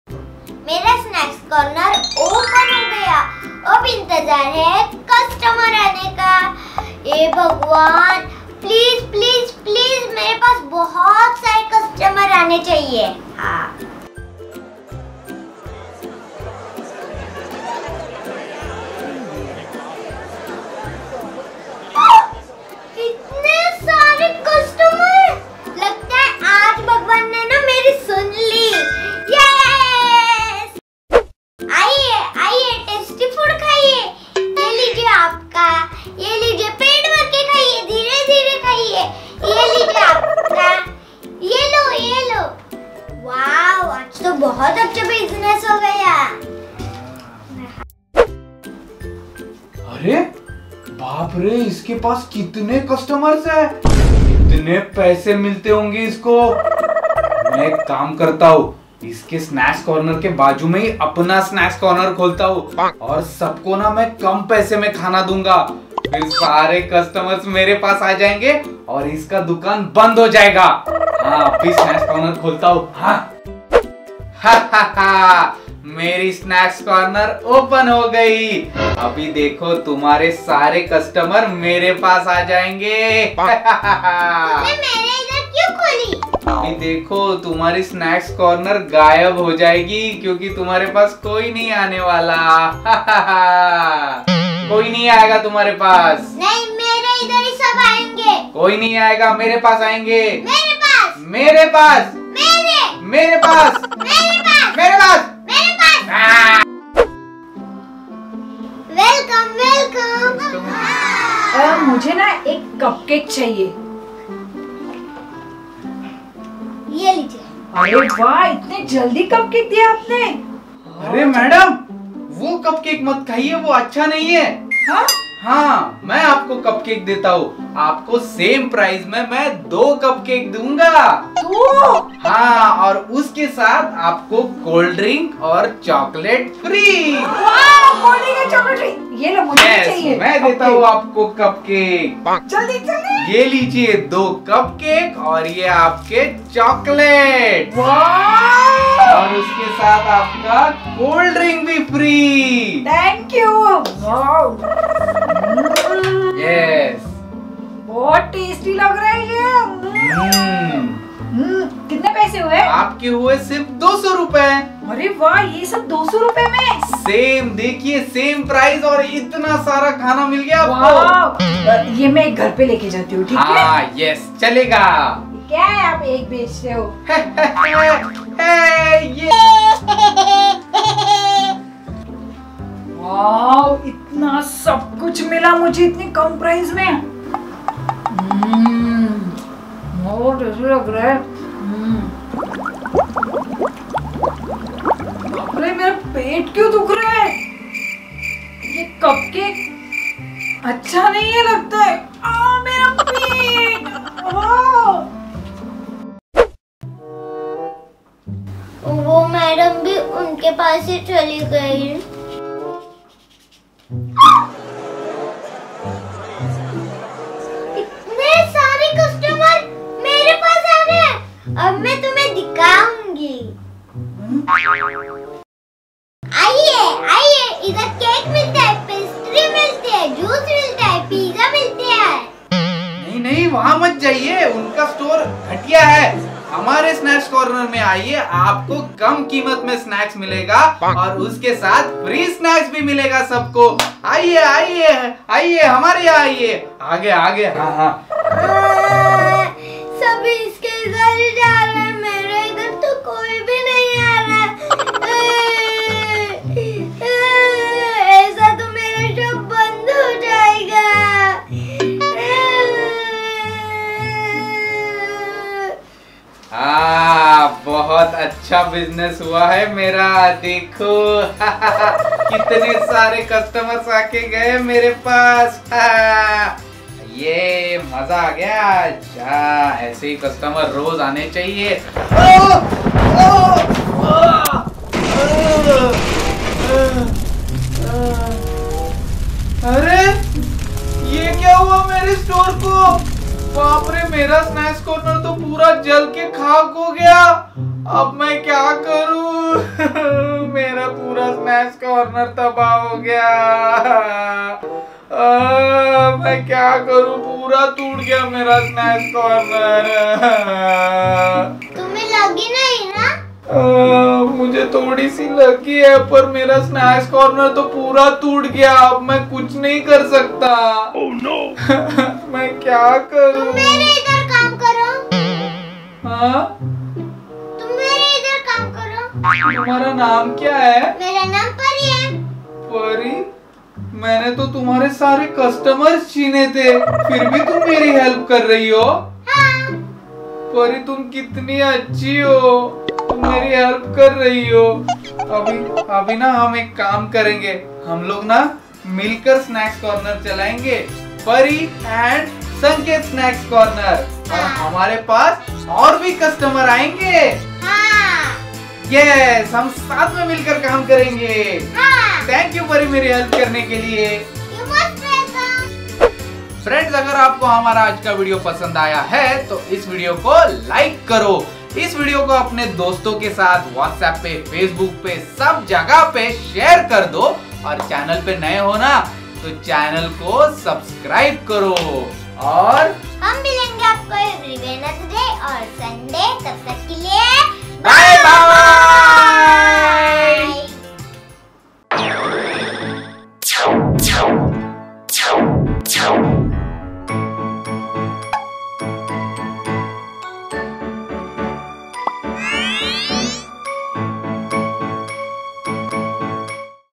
Who Is Better Cook ? Snacks Corner War Moral Story | Pari's Lifestyle
In today's video pari have opened new shop and sanket is trying to close her shop by copying her store idea and stealing her customers.
We hope you guys love this video.

Picture credits- Freepik.com

Toys link
1. Magic Wand
2. Makeup Set
3. Nail Art Kit
4. Doctor Set
5. Kitchen Set
6. Fruit mould
7. animal mould
8. smiley mould